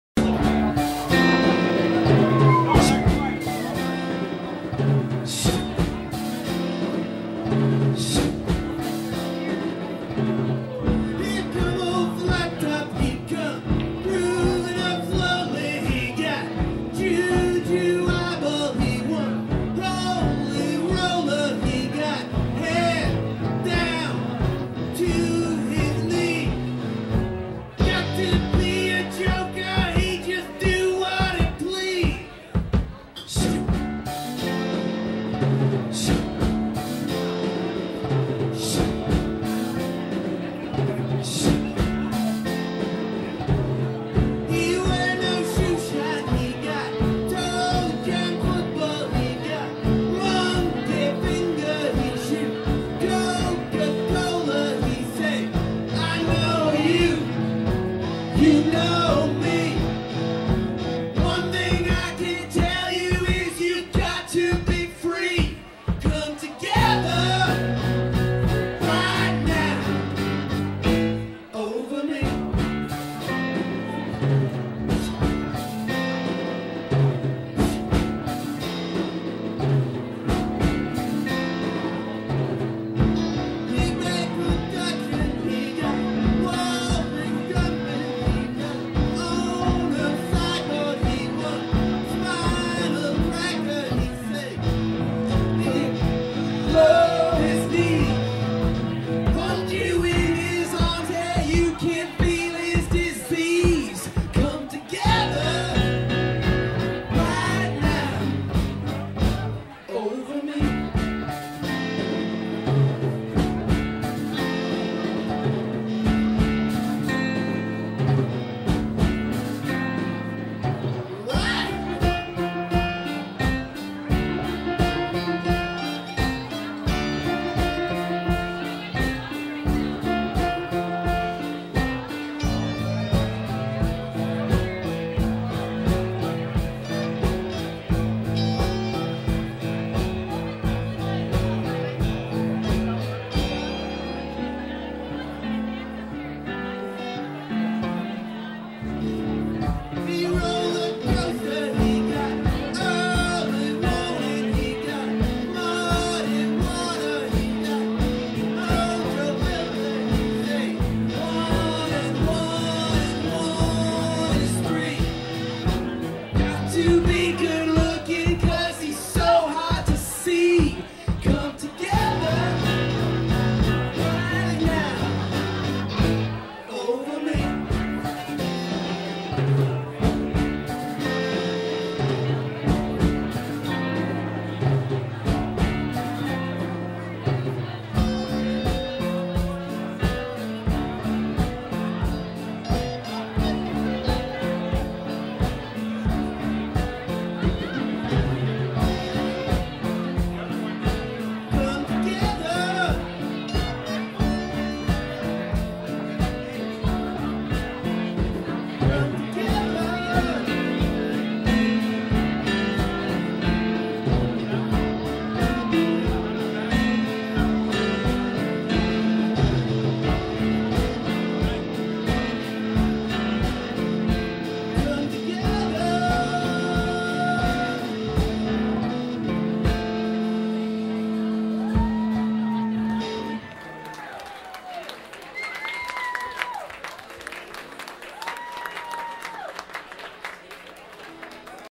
Walrus Gumbo - Song 4
Walrus Gumbo presented by Twin Town Tributes at The Whiskey Junction in Minneapolis, May 17, 2013.